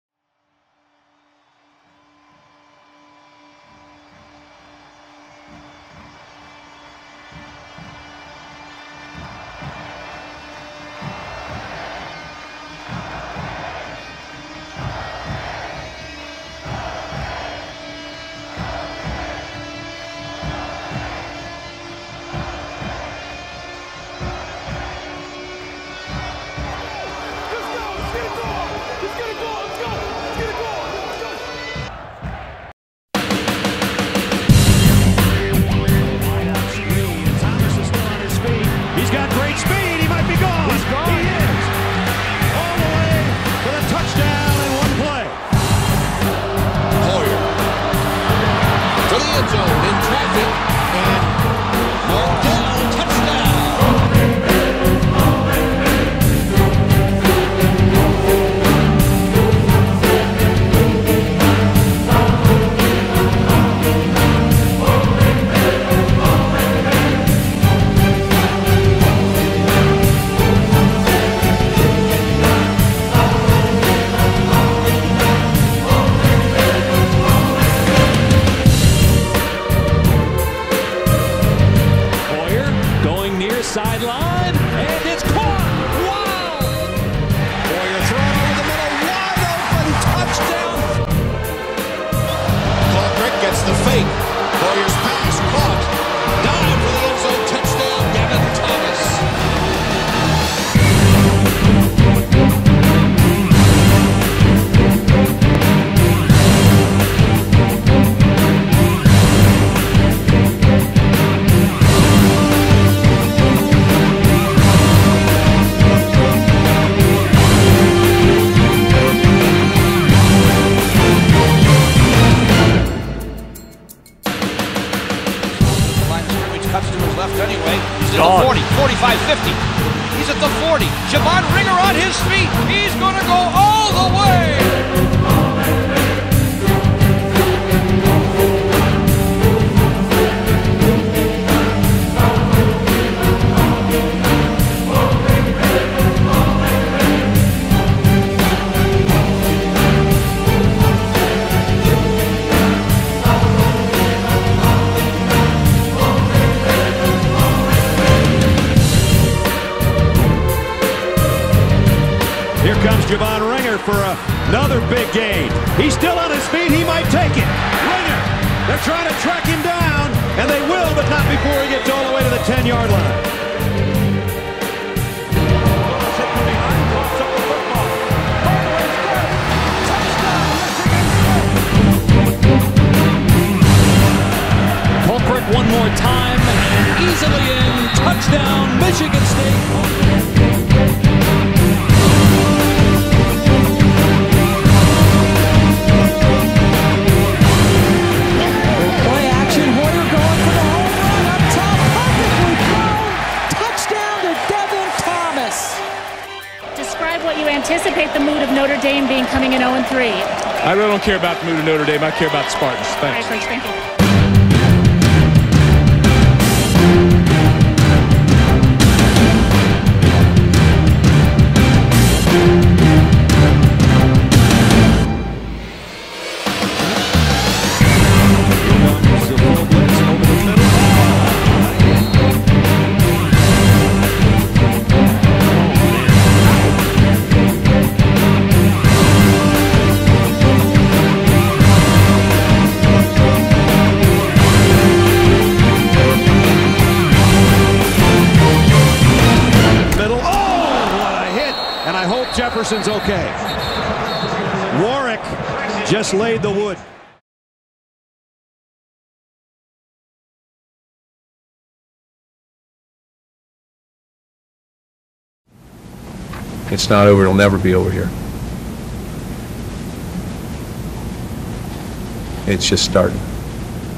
2007 Michigan State Football - Part 1
Highlights of the 2007 season of Spartan Football.  First in a 3 video series.